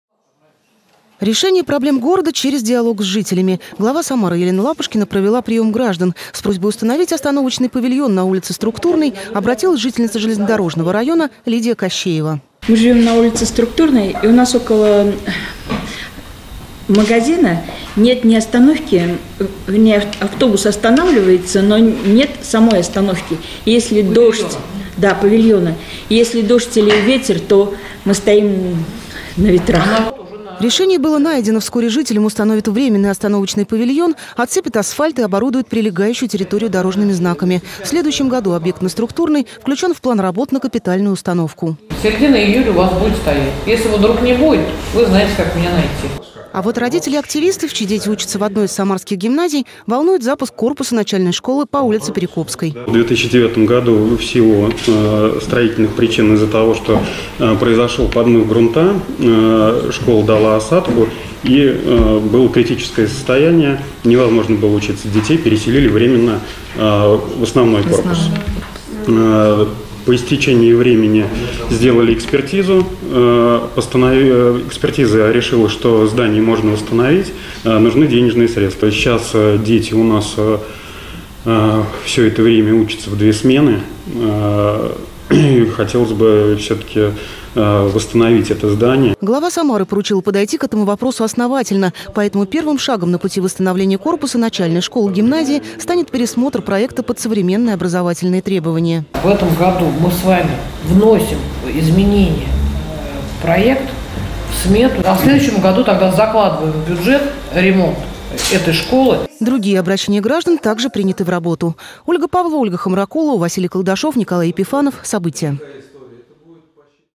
Глава Самары Елена Лапушкина провела прием граждан
От установки остановочного павильона до восстановления школ. Глава Самары Елена Лапушкина провела личный прием граждан. Что волнует жителей города, — расскажут наши корреспонденты.